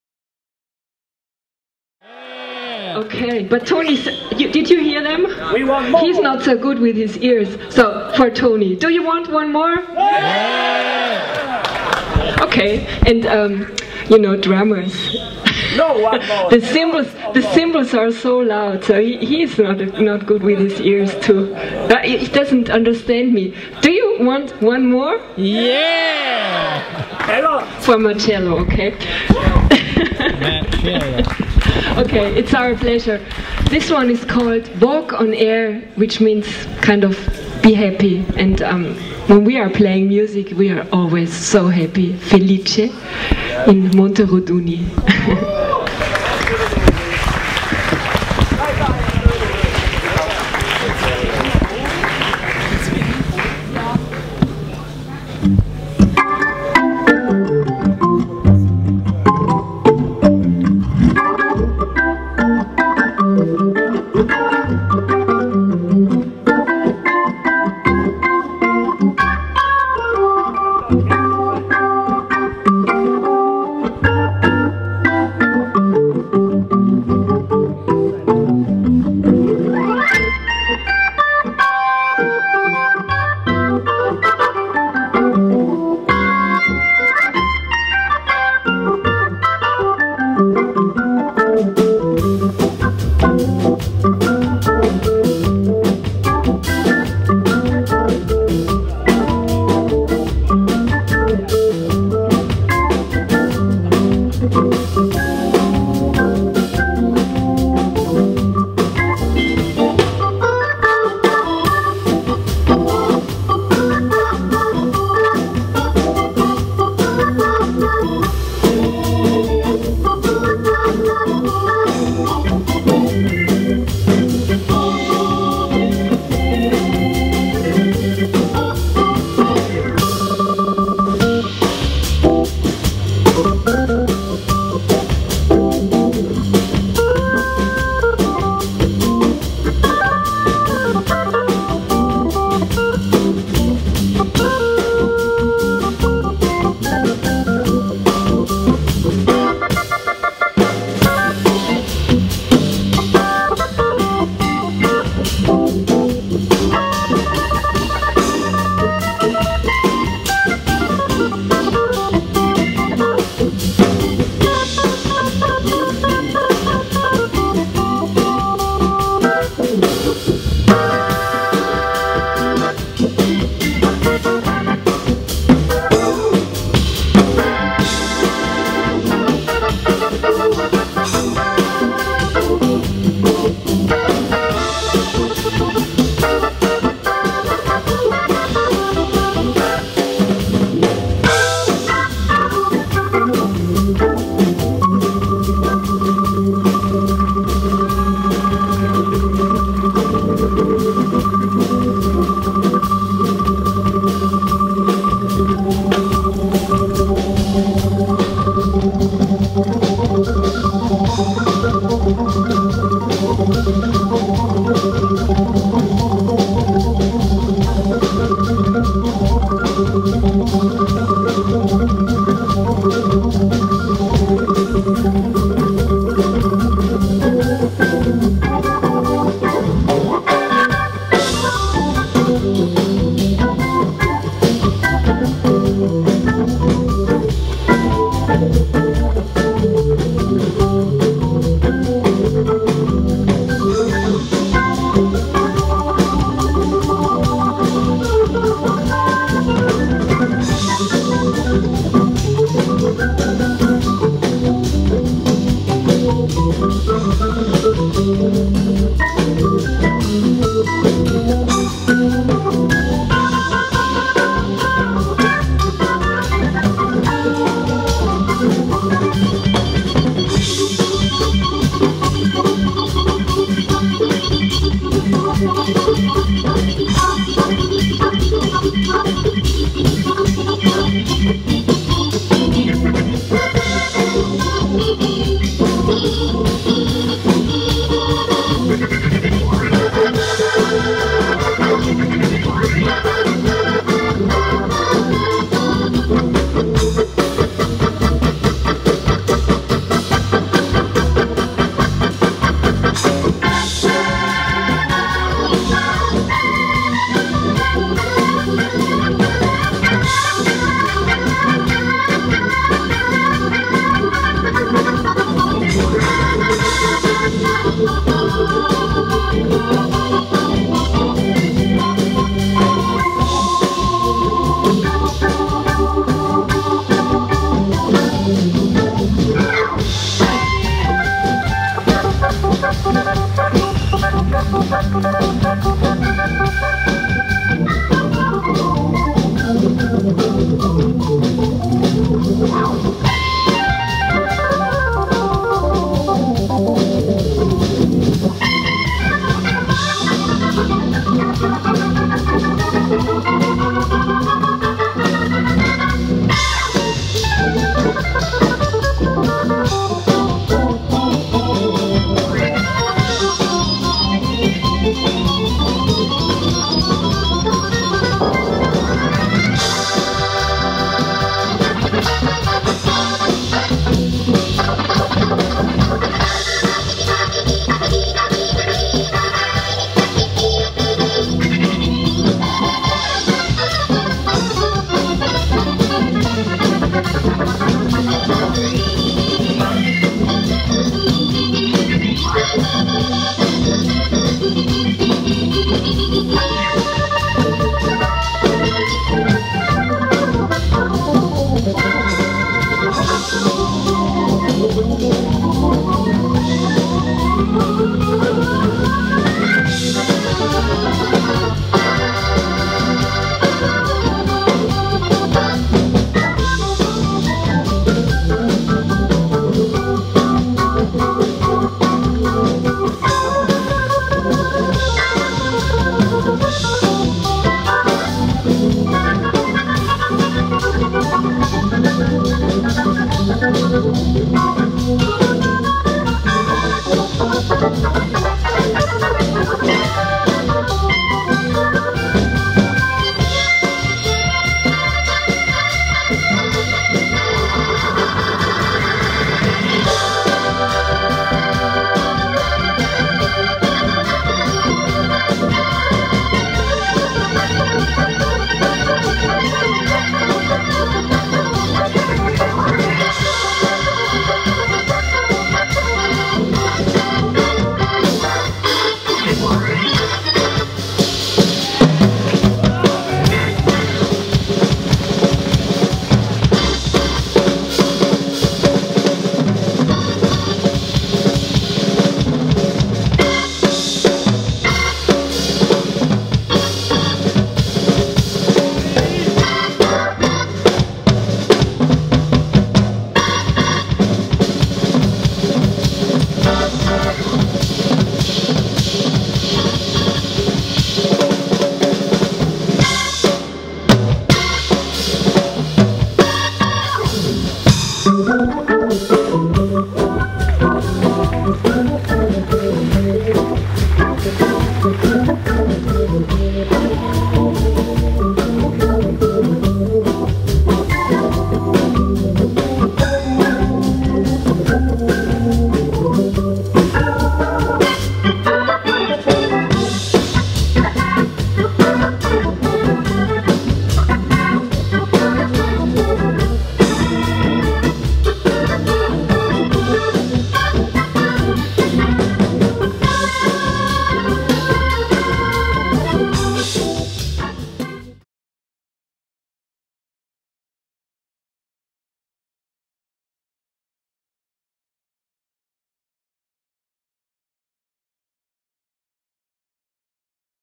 Tony Monaco e Barbara Dennerlein
Eddie Lang Jazz Festival - 2011 - Moteroduni - Isernia - Italy

Barbara Dennerlein e Tony Monaco chiudono una splendida serata dedicata all'Organo Hammond.